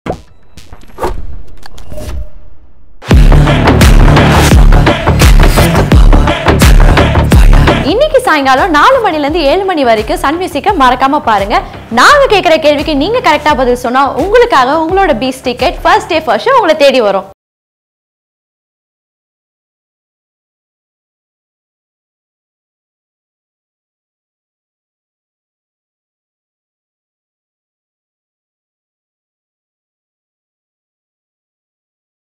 Beast 1st Day 1st Show pakka ready ah? | Beat Ticket Contest - Promo | Sun Music
Are you all ready to watch the beast movie on 1st day? Here is the super exciting "Beast Ticket Contest" for you. Watch Sun Music shows and answer our simple questions and win the prize.

Beast ticket contest | April 7 - April 11 | 4 PM - 7 PM